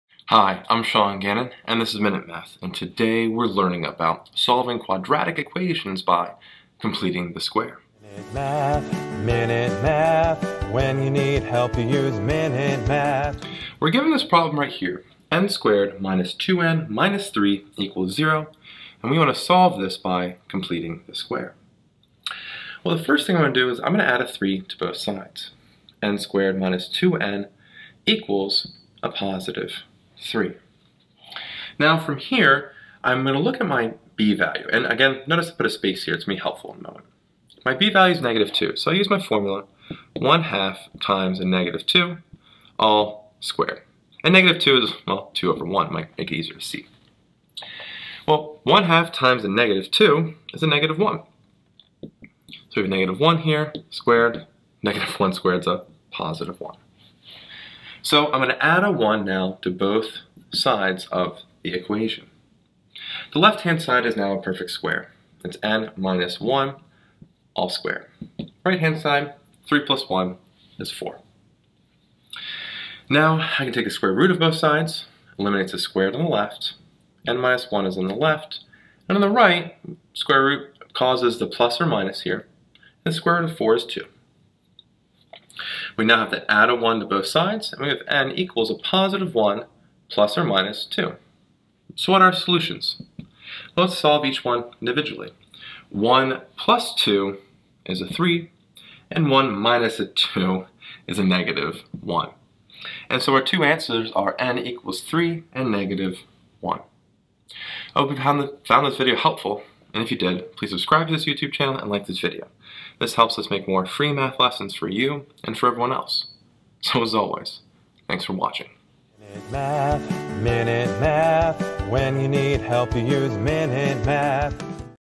Solve n^2-2n-3=0 by Completing the Square
Solve n^2-2n-3=0 by Completing the Square. We learn how to solve n^2-2n-3=0 by completing the square. We learn how to solve a quadratic equation by completing the square. This is a great introduction into quadratic equations where we learn how to complete the square to solve a quadratic equation. This is under the topic of Solving Quadratic Equations by Completing the Square with this free online math video lesson.